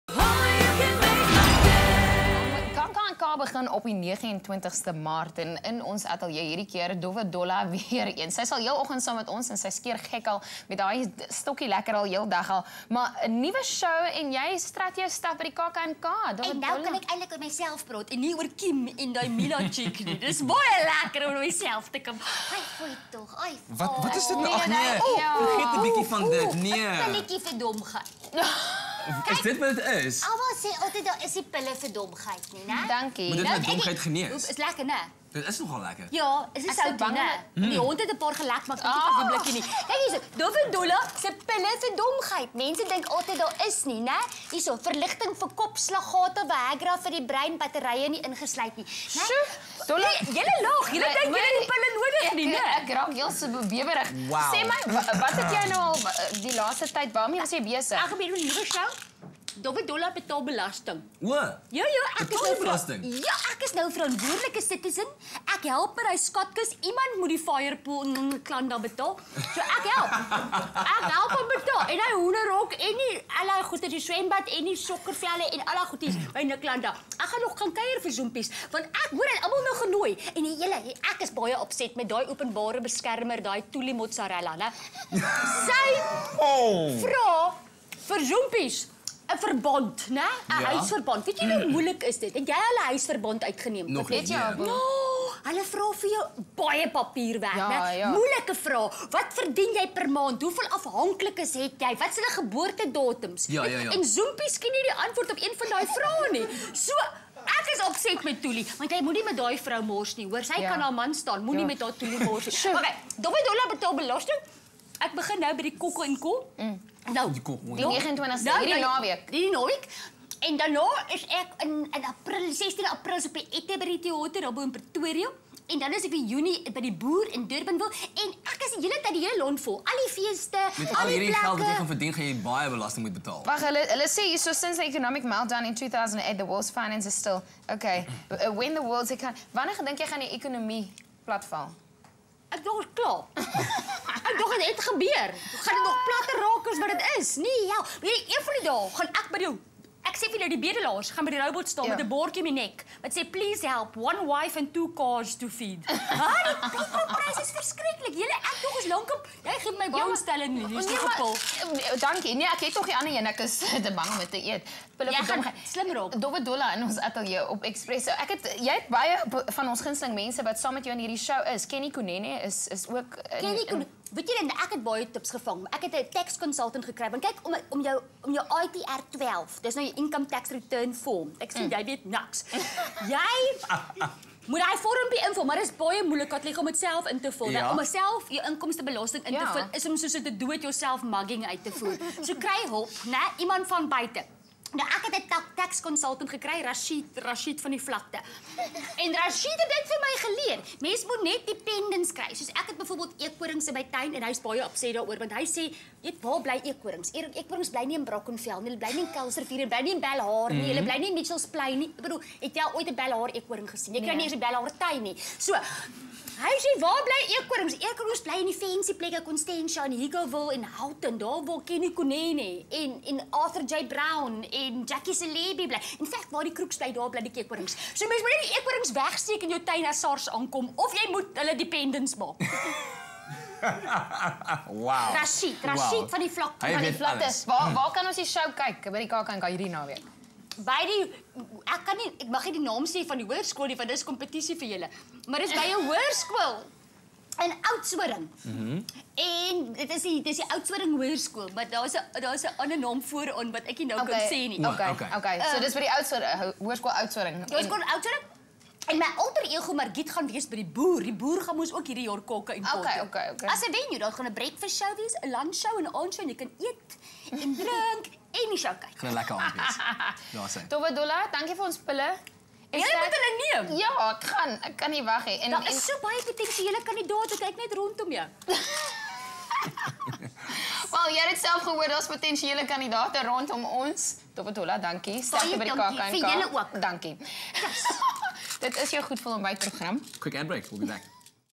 Dowwe Dolla
We're thrilled to have back with us in studio, Margit Meyer Rödenbeck, AKA "Dowwe Dolle", who is on a nationwide tour with her brand new show and will be strutting her stuff at this year's KKNK from the 29th of March.